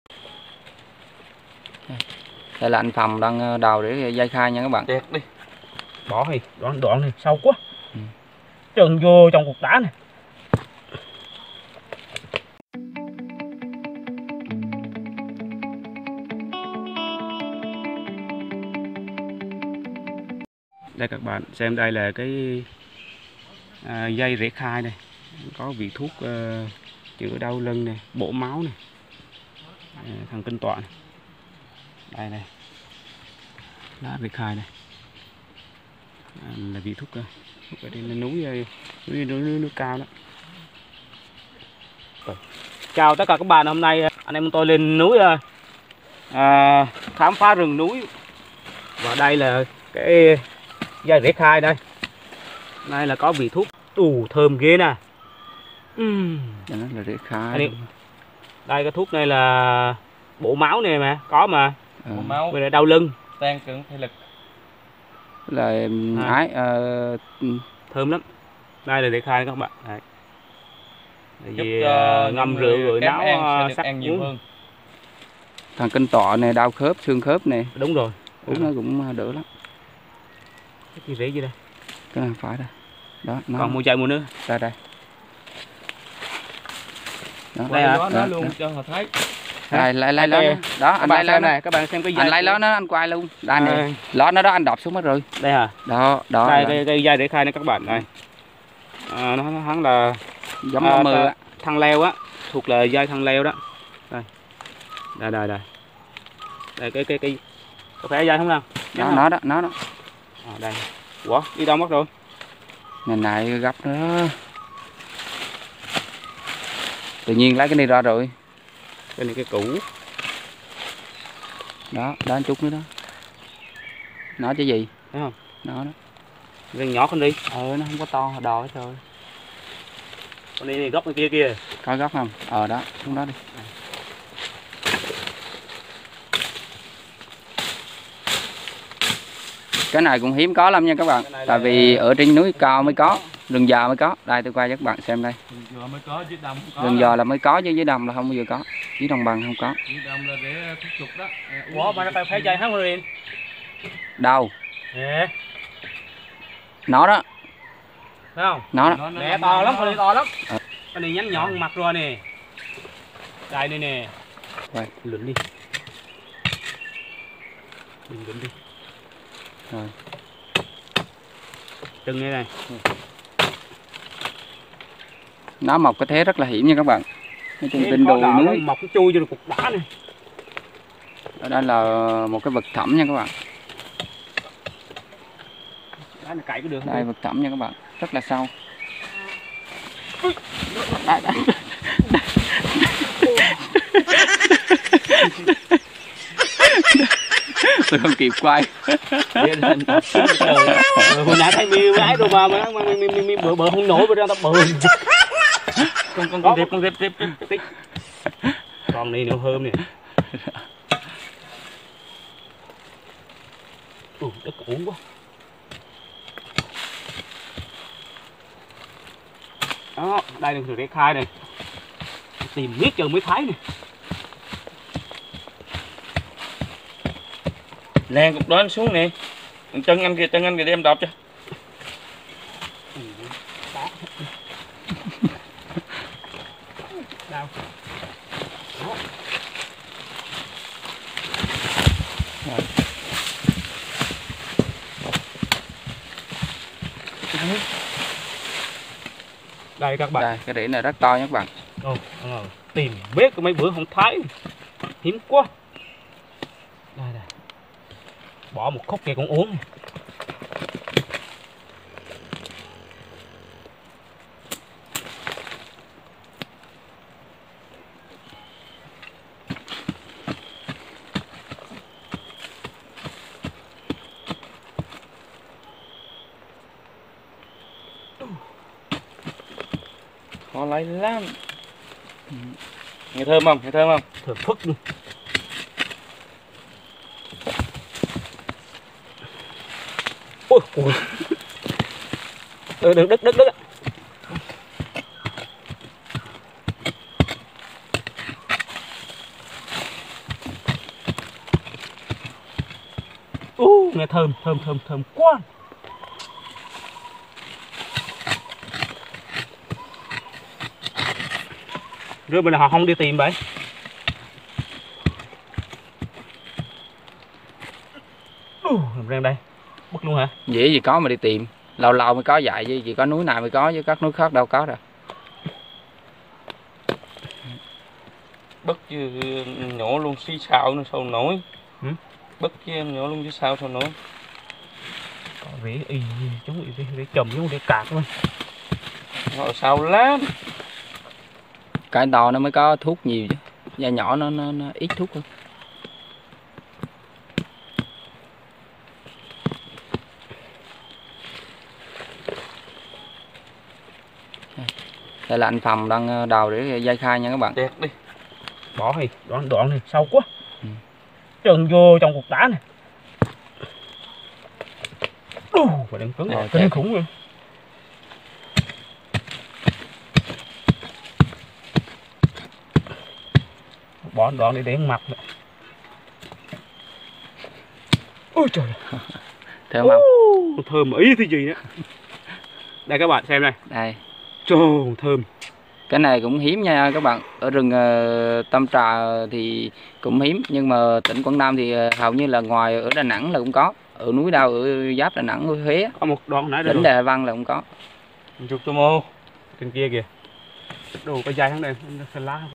DÂY RỄ KHAI - VỊ THUỐC RỪNG NÚI / BÍ ẨN RỪNG NÚI
Dây rễ khai vị thuốc rừng núi. Đây là thuốc nam ở trên rừng, loại thuốc quý hiếm.
Dây rễ khai dạng thân leo, có vị thuốc trị bệnh đau lưng, kích thích ăn uống.
Dây rễ khai chỉ có trên núi cao, ở chủ yếu chỗ xen đá, vách núi,...
   Chân thành cảm ơn các bạn đã quan tâm theo dõi.